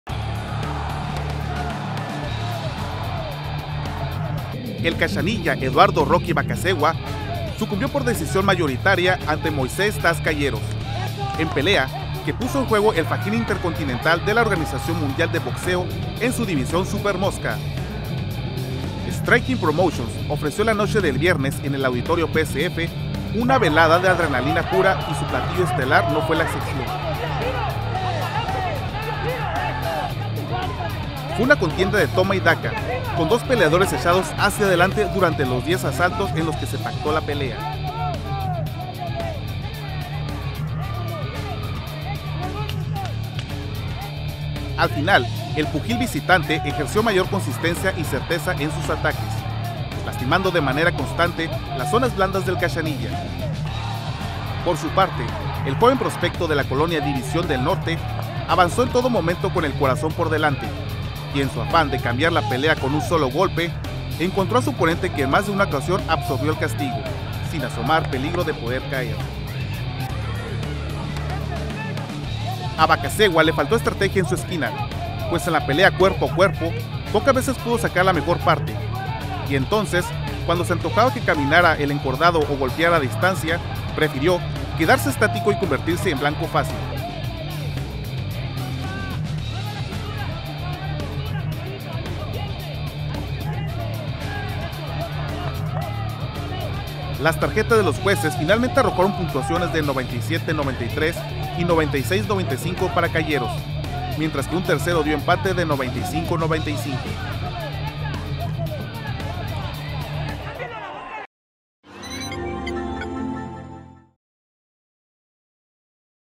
Eduardo Bacasegua en misión fallida
El cachanilla Eduardo ‘Rocky’ Bacasegua (13-4-2, 5 Ko´s) sucumbió por decisión mayoritaria ante Moisés ‘Taz’ Calleros (32-9-1, 17 Ko´s), en pelea que puso en juego el fajín Intercontinental de la Organización Mundial de Boxeo (OMB) en su división Supermosca.

Video: Arturo Ceceña